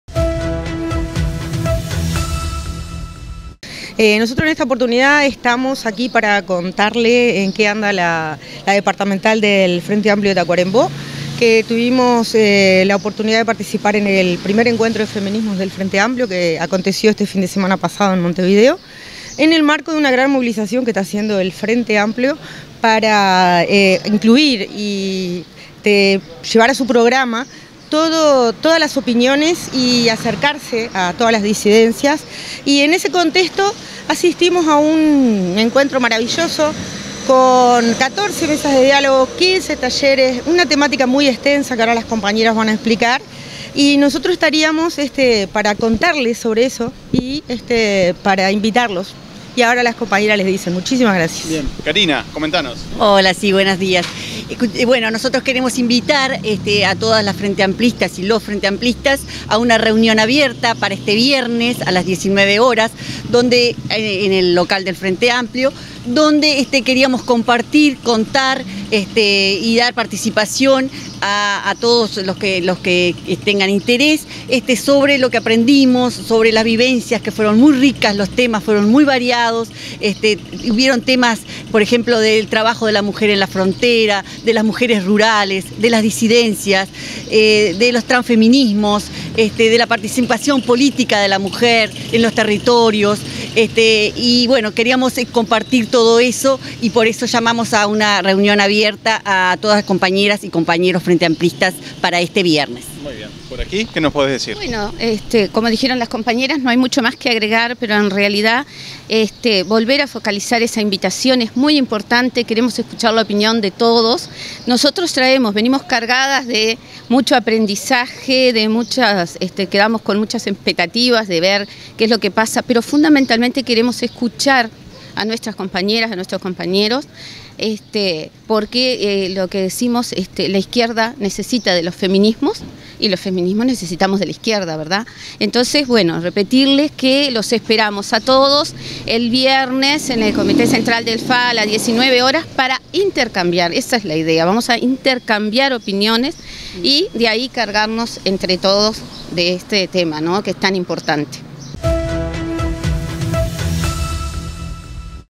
Reflexiones iniciales sobre el primer encuentro de feminismos
Así se denomina la charla abierta, que tendrá lugar este viernes a las 19:00 horas, en el local central del Frente Amplio.